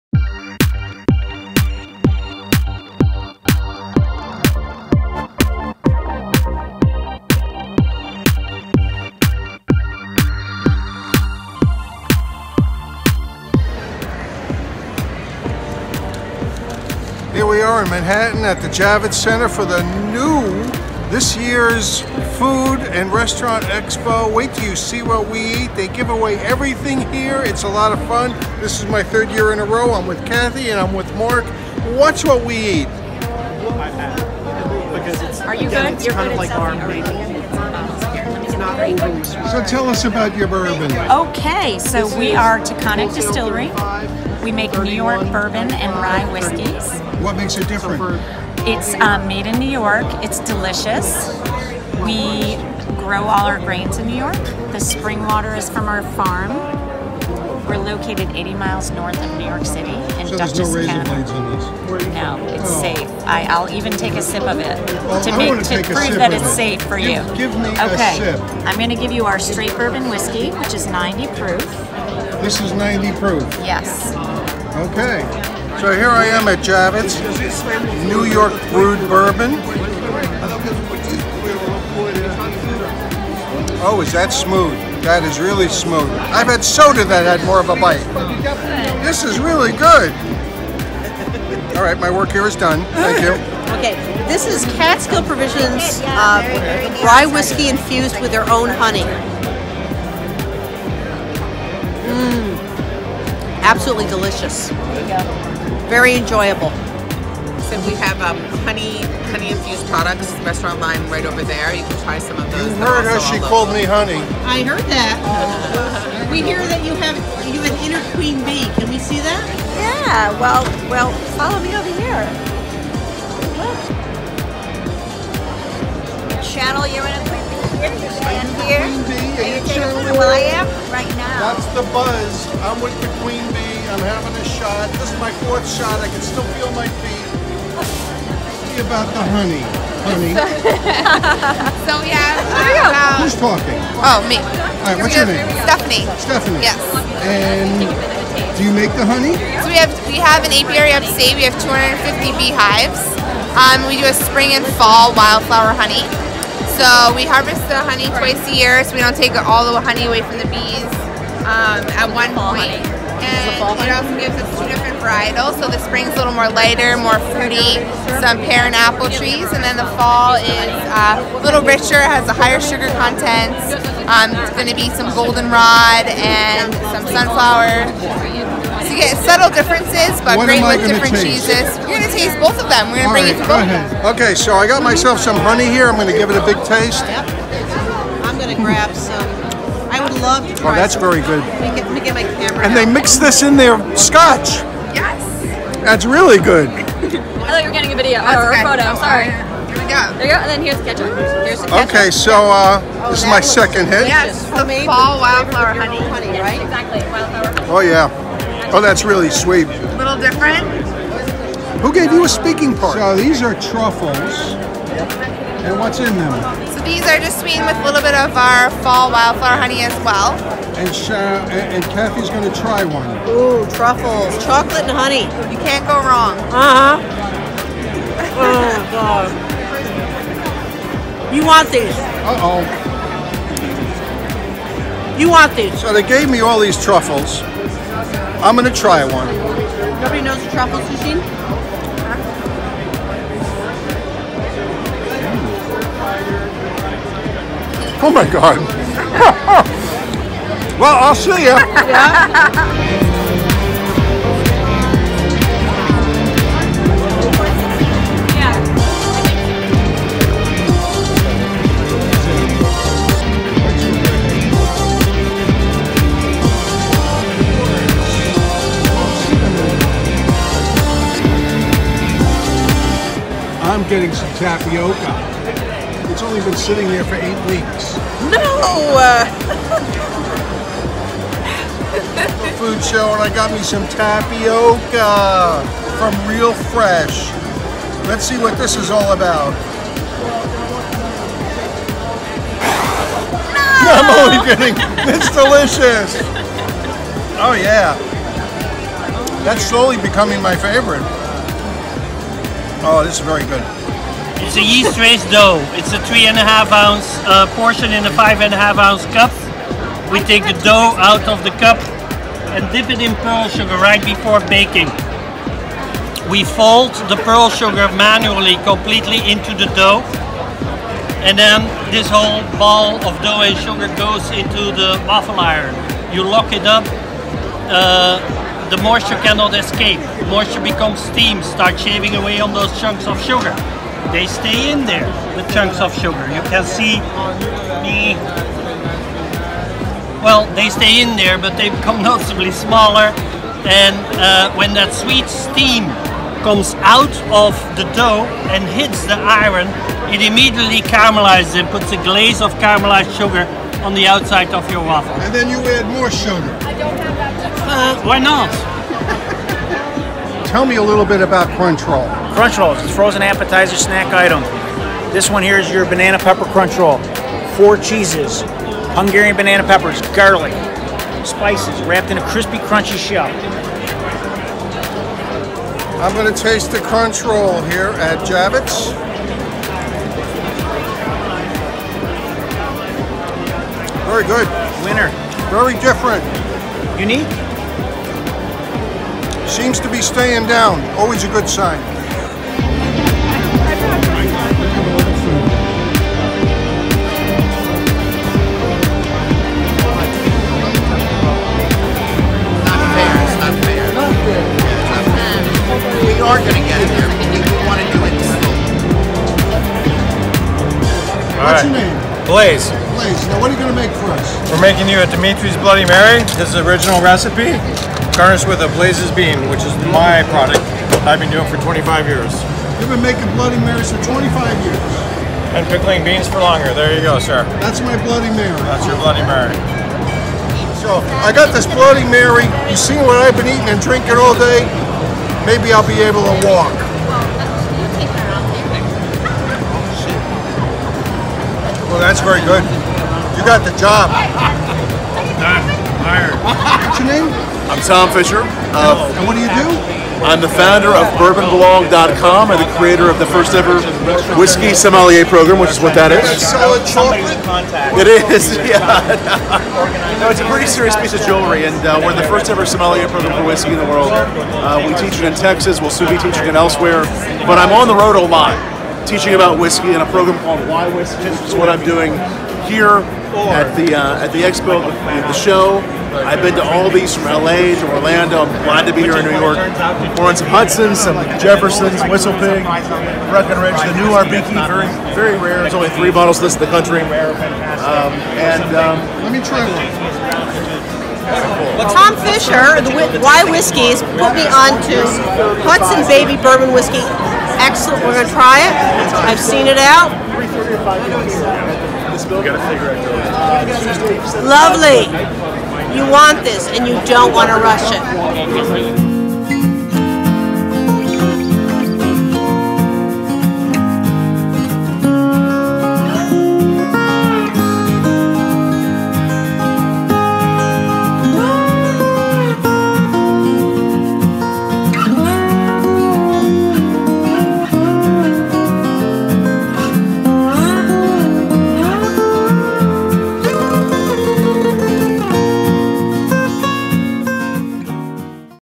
2018 International Food And Restaurant Show At Jacob Davits Center In New York City
My good buddy Marc Daichman gets tickets for this event and I join him in NY for the fun. This year again, Kathy from Savory Pixels joined us for this event. Put this on your calendar for next year